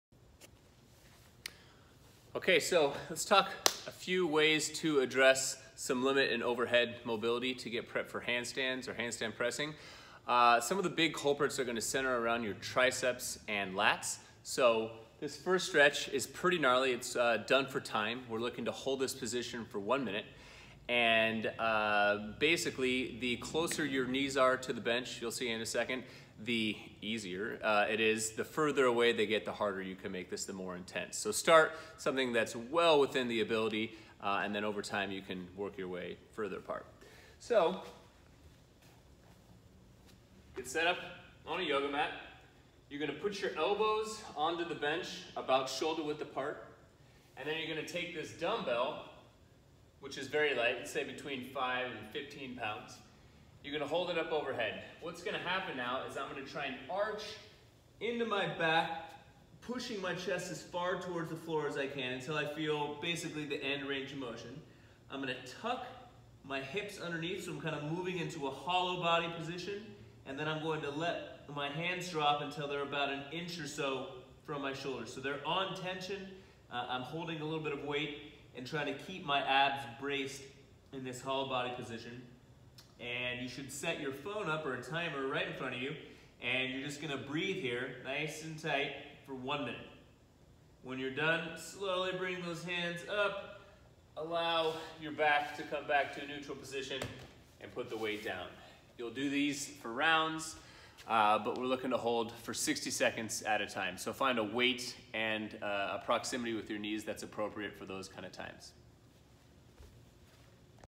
Mobility - Lat-tri stretch
Do you have 12 extra minutes before or after class?

Would you like to use those 12 minutes to improve your overhead mobility?

Try this...

Three rounds through:

1:00 Passive hang

1:00 Foam rolling of lats on each side

1:00 Lat-tricep insertion stretch

3x/week for 30 days, boom!